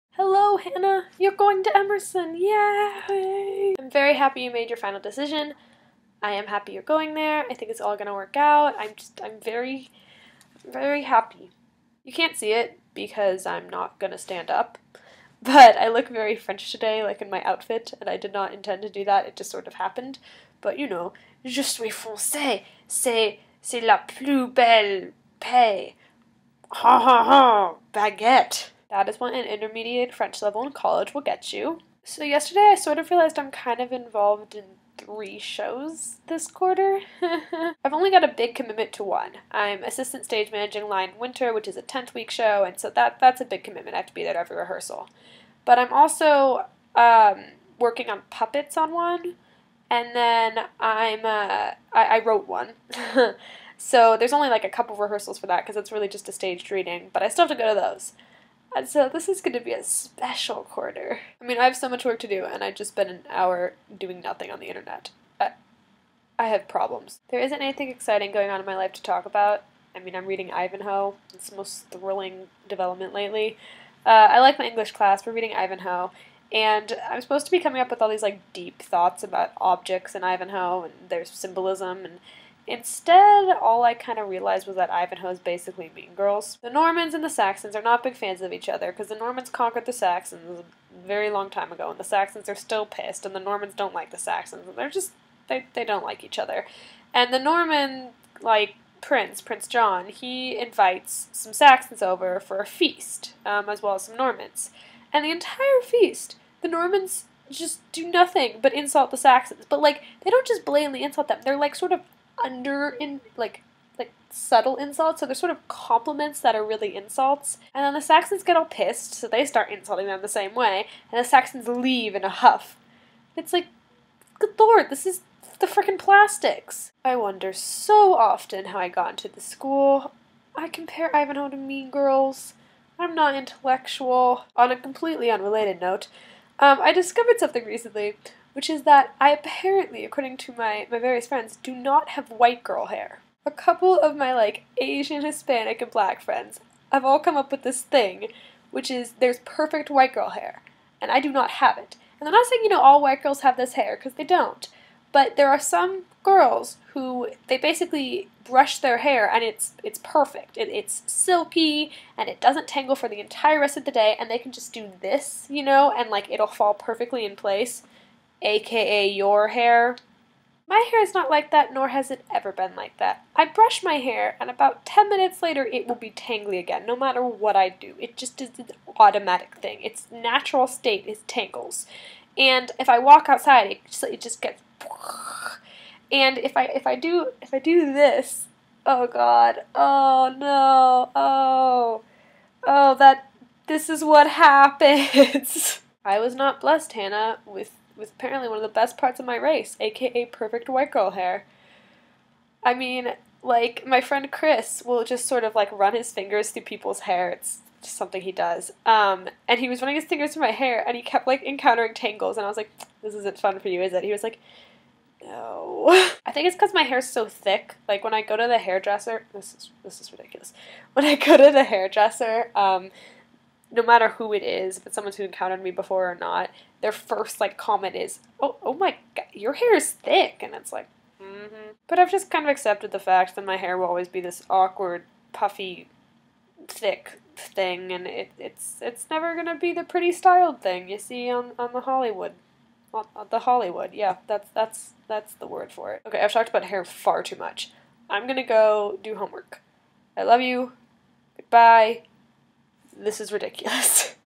Too many shows! Ivanhoe and Meangirls! Perfect white girl hair!
In which Caety congratulates Hannah on her choice of Emerson, discusses how she realized she's involved in three shows this quarter and that is ridiculous, and then explains the concept of "perfect white girl hair" and how she very much does not have it.